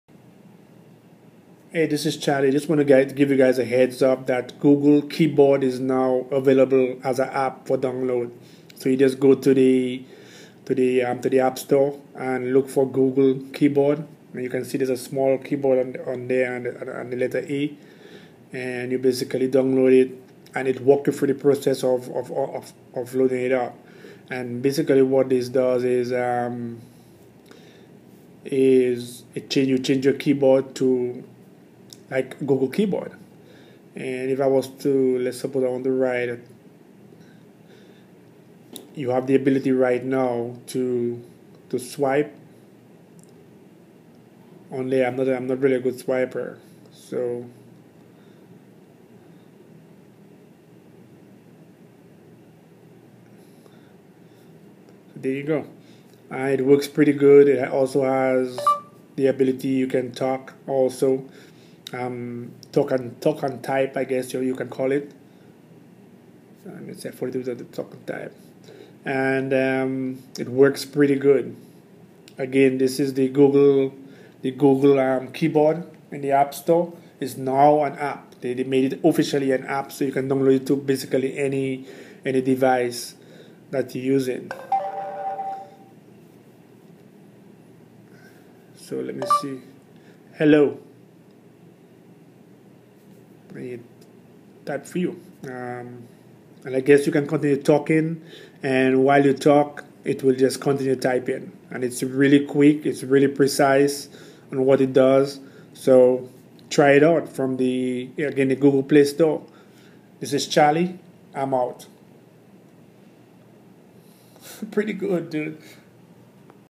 GOOGLE KEYBOARD APP WITH GESTURE AND VOICE TO TYPING
• Gesture Typing with dynamic floating preview
• Voice Typing
• Next-word suggestions and current-word completions
• Dictionaries for 26 languages
• Advanced keyboard layouts
• Works across your Android devices (tablets and phones)